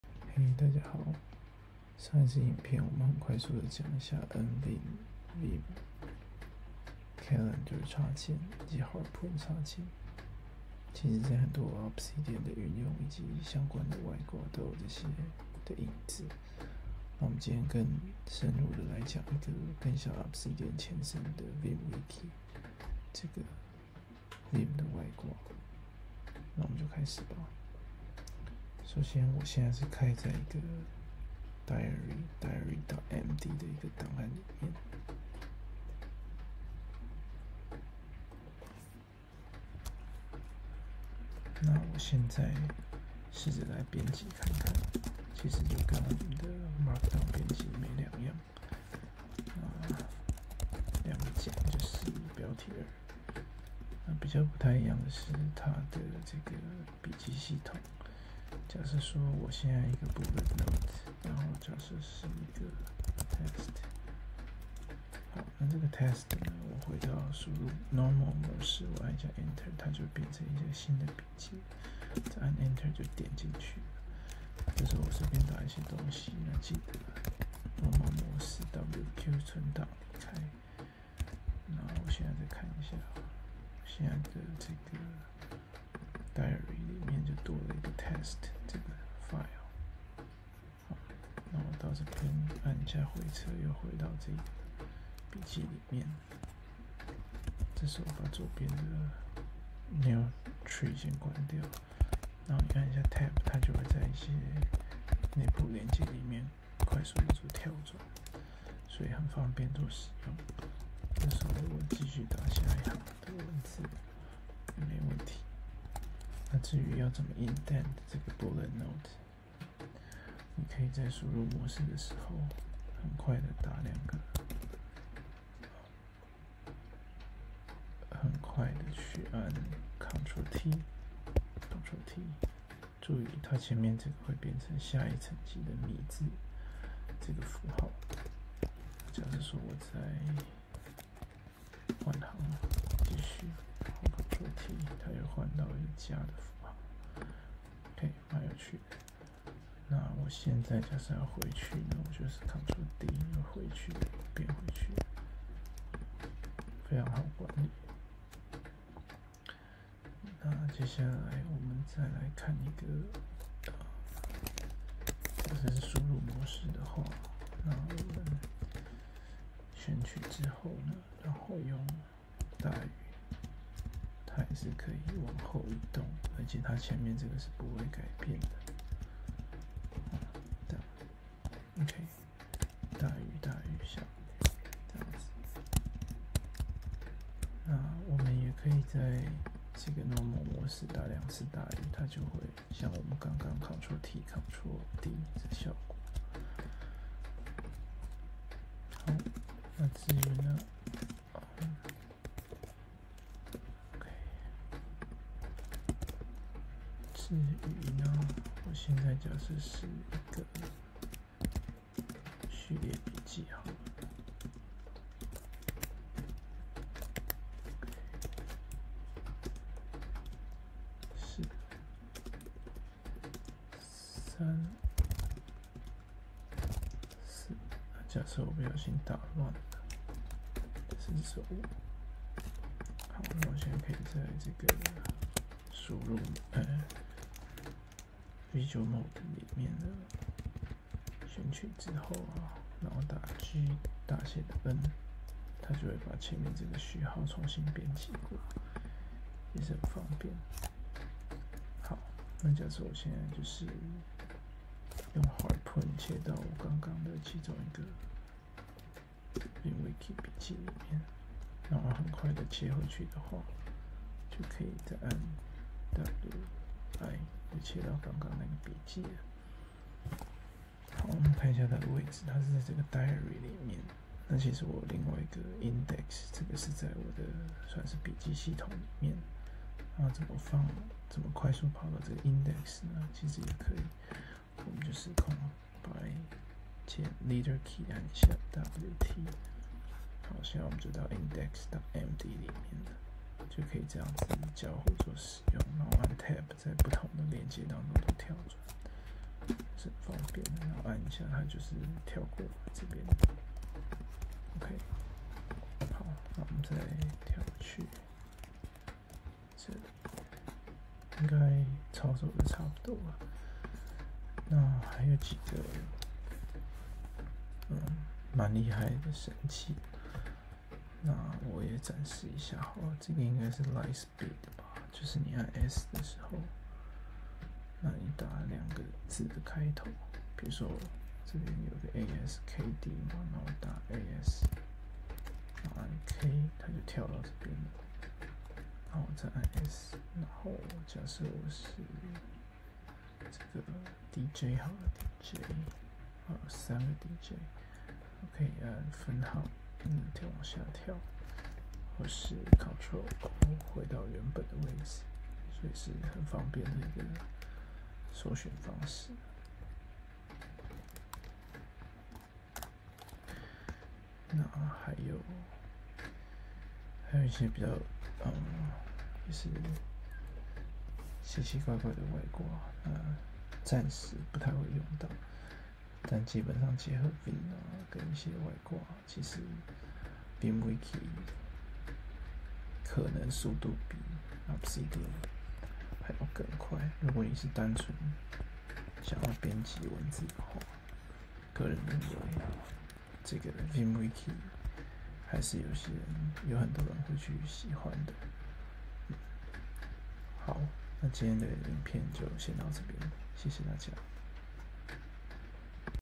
🙂️ 系列之二 - Vimwiki 的使用介紹
Vimwiki 的使用操作，為接下來 obsidian.nvim 做準備